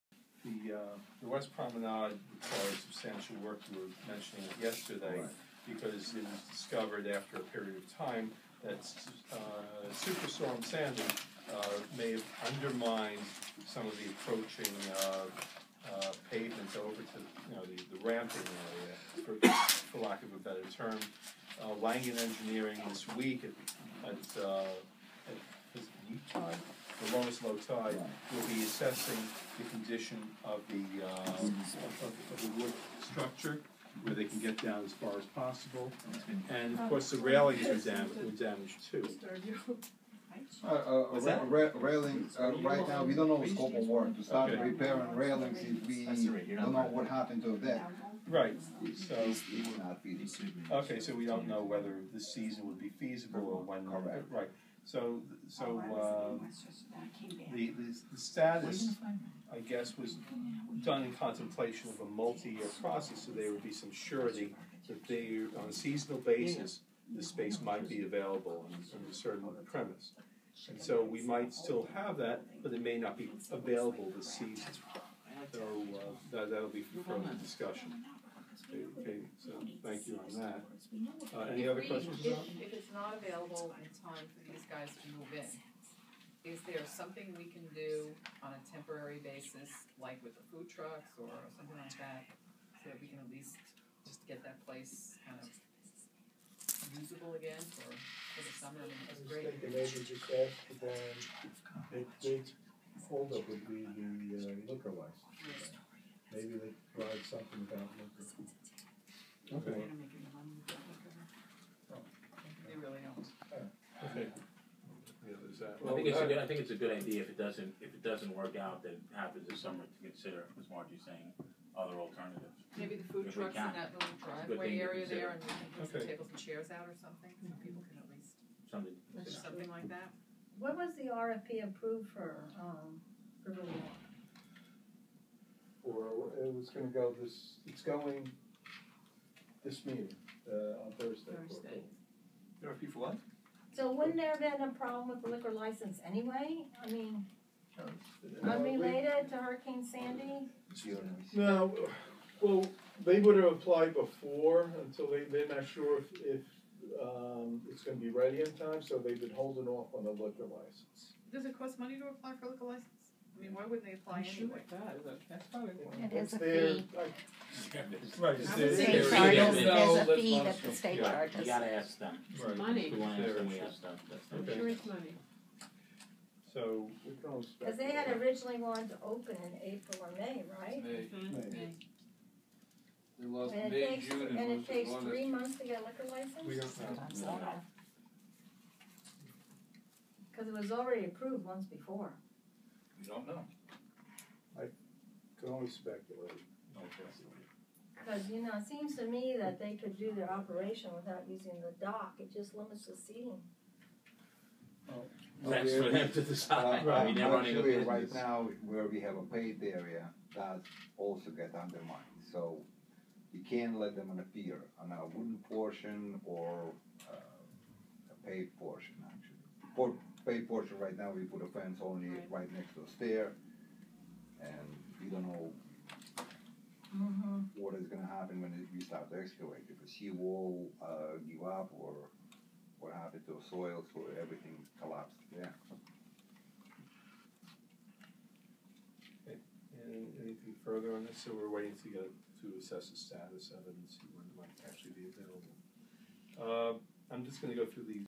Hurricane Sandy Damage To Subway Pier and Future Of Waterfront Restaurant
April 1 Roosevelt Island Operating Corp Operations Committee meeting discussion of damage to Subway Pier from Hurricane Sandy and future operations of waterfront restaurant.